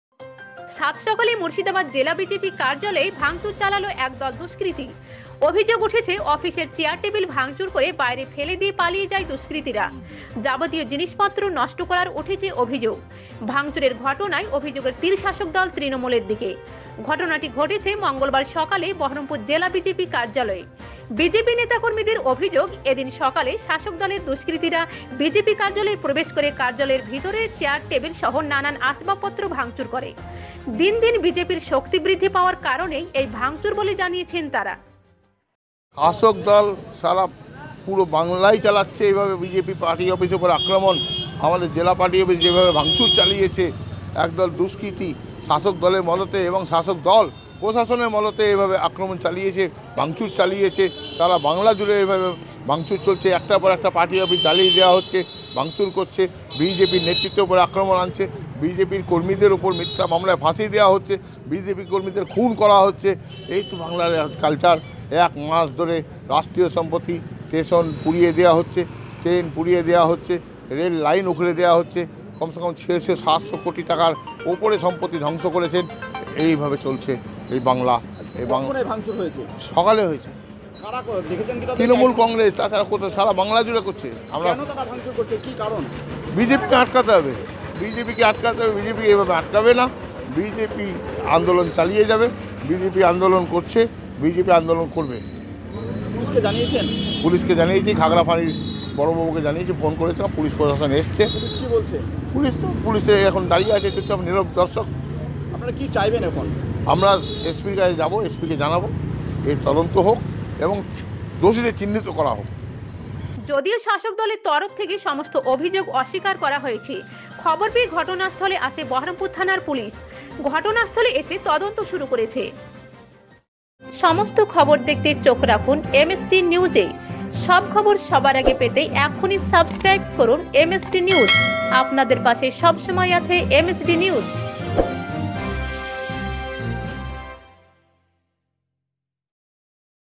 মুর্শিদাবাদ জেলা বিজেপি কার্যালয়ে দুষ্কৃতীদের তান্ডব
সাত সকালে মুর্শিদাবাদ জেলা বিজেপি কার্যালয়ে ব্যাপক ভাঙচুর অভিযোগের তীর শাসকদল তৃণমূলের দুস্কৃতিদের দিকে। ঘটনাটি ঘটেছে মঙ্গলবার সকালে বহরমপুর জেলা বিজেপি কার্যালয়ে। বিজেপি নেতা কর্মীদের অভিযোগ এদিন সকালে শাসকদলের দুস্কৃতিরা বিজেপি কার্যালয়ে প্রবেশ করে কার্যালয়ের ভিতরে চেয়ার, টেবিল সহ নানান আসবাবপত্র ভাঙচুর করে। দিন দিন বিজেপির শক্তি বৃদ্ধি পাওয়ার কারনেই এই ভাঙচুর বলে জানিয়েছেন তারা। বহরমপুর থানার পুলিস ঘটনাস্থলে এসে তদন্ত শুরু করেছে।